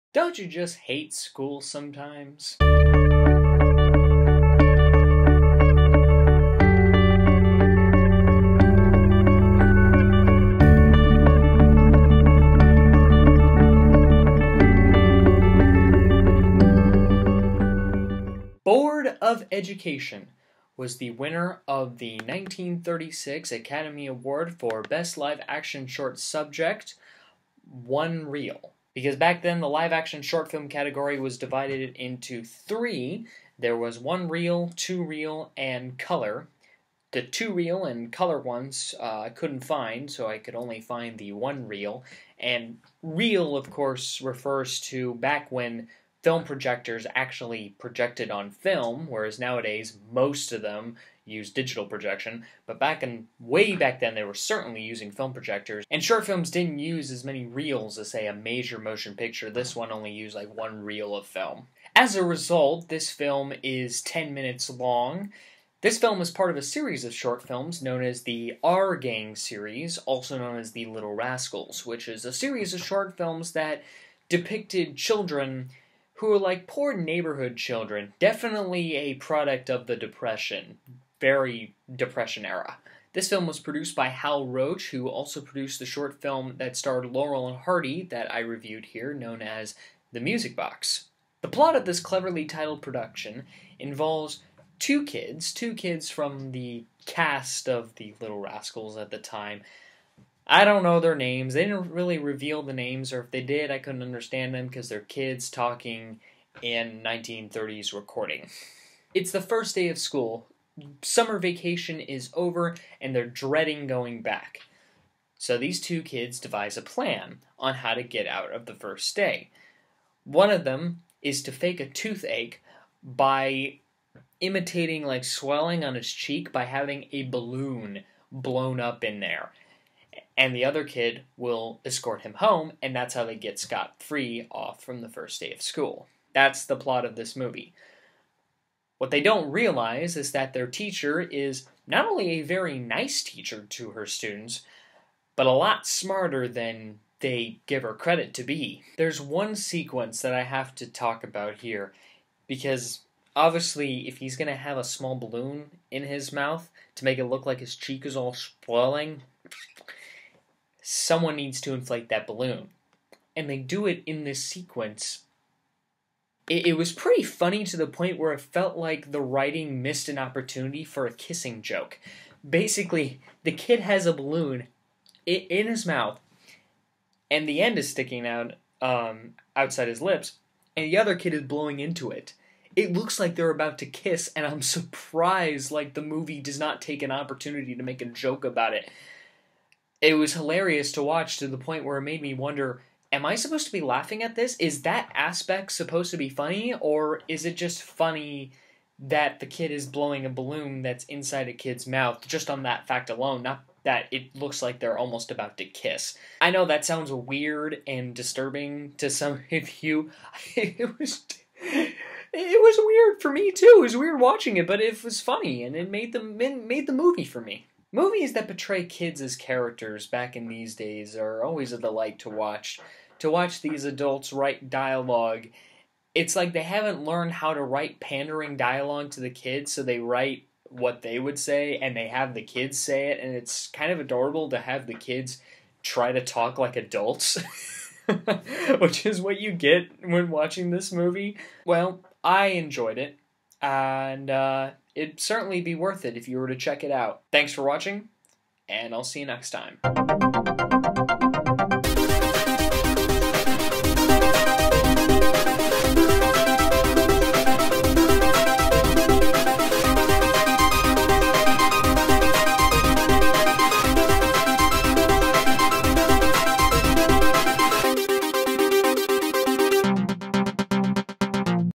"Bored of Education" review
Some days, you just don't want to go to school... until something actually happens there.